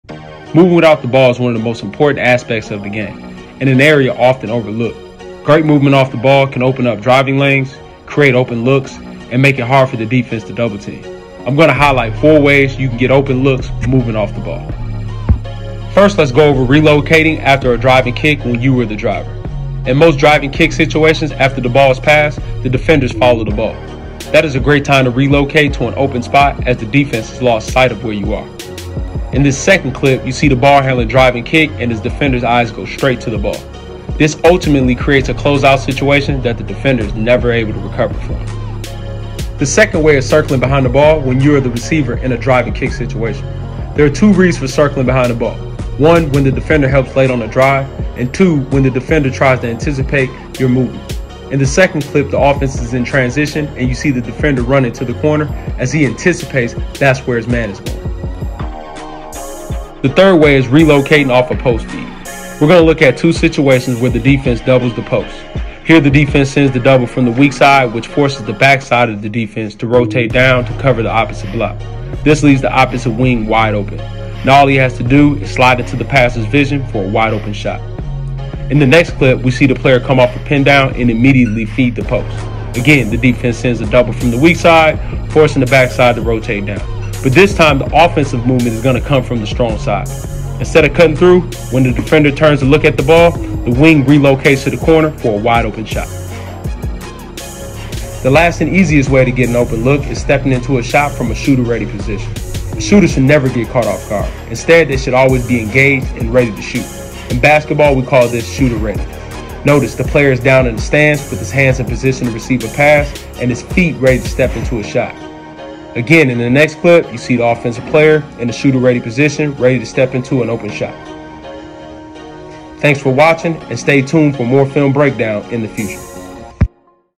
Film Breakdown: 4 Ways to Move without the Ball to Get Open Looks
Want more open looks? Watch Coach Nick breakdown 4 ways you can move without the ball to get more open looks!

1) Relocate after Drive & Kick
2) Circle Behind teammate
3) Relocate after Post Feed
4) Shooter Ready